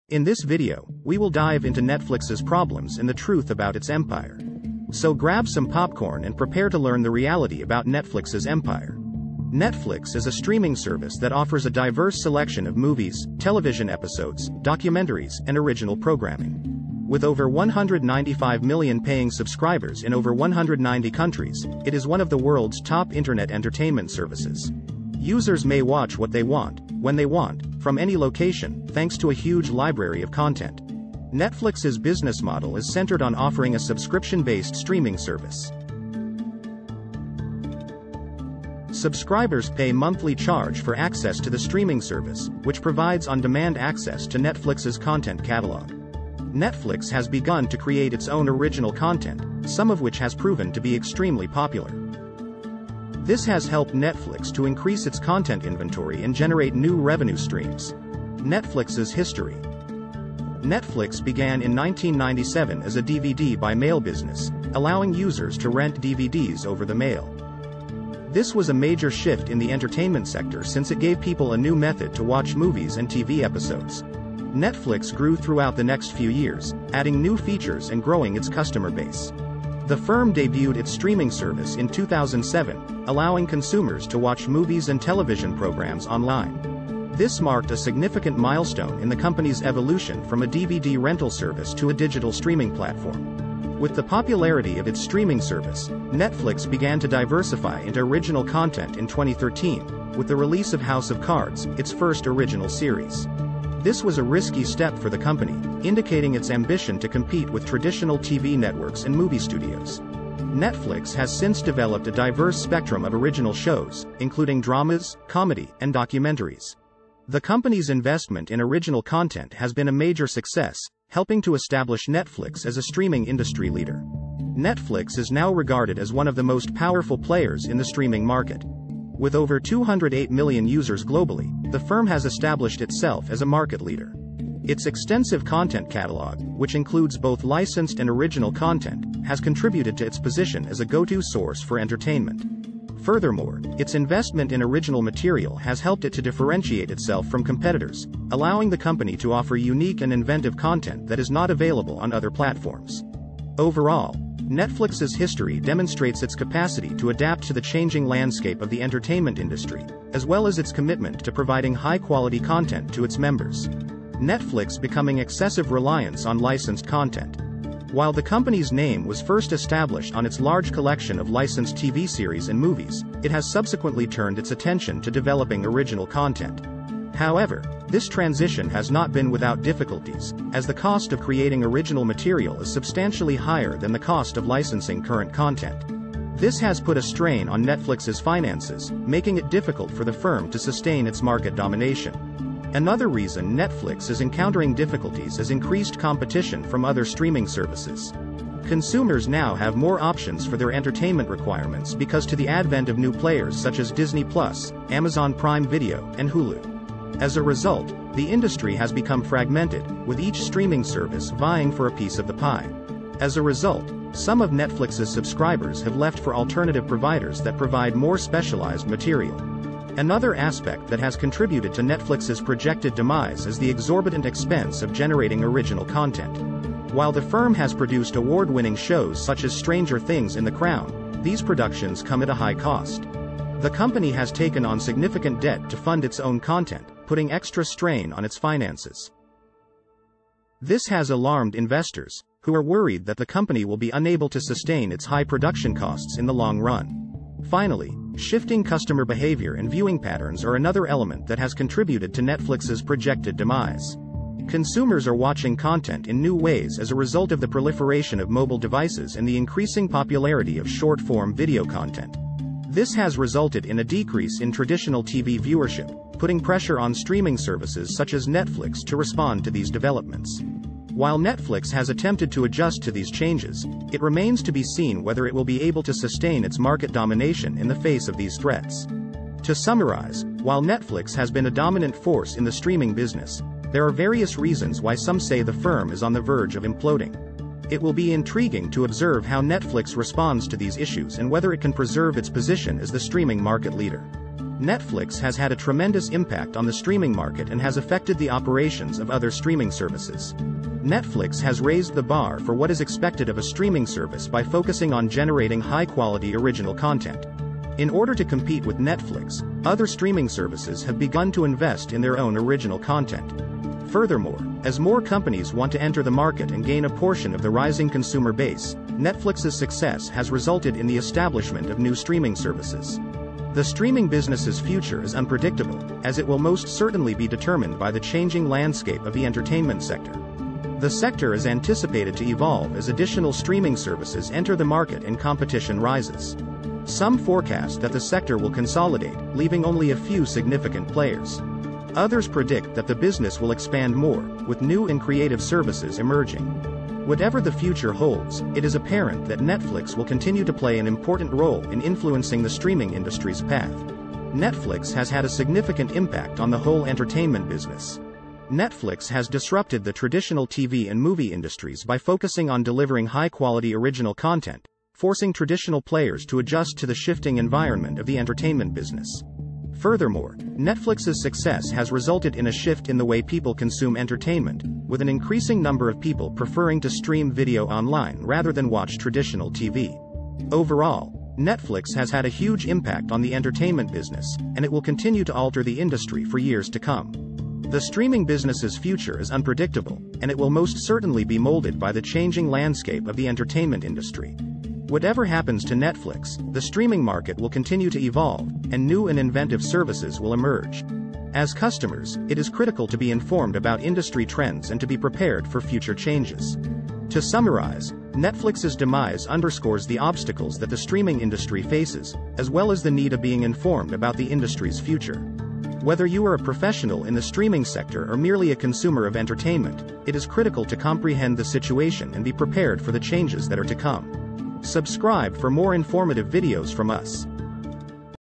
The Truth About Why Netflix Is Collapsing
Netflix is a popular streaming service and for a good reason - it has a huge library of movies and TV shows to choose from. But lately, Netflix has been experiencing some problems.

Some people are saying that Netflix is collapsing due to the increasing competition from streaming services like Hulu and Amazon Prime. But is this really true? In this video, we'll look at the evidence and try to answer the question - is Netflix collapsing?

p/s: Netflix's password sharing policy also coming soon.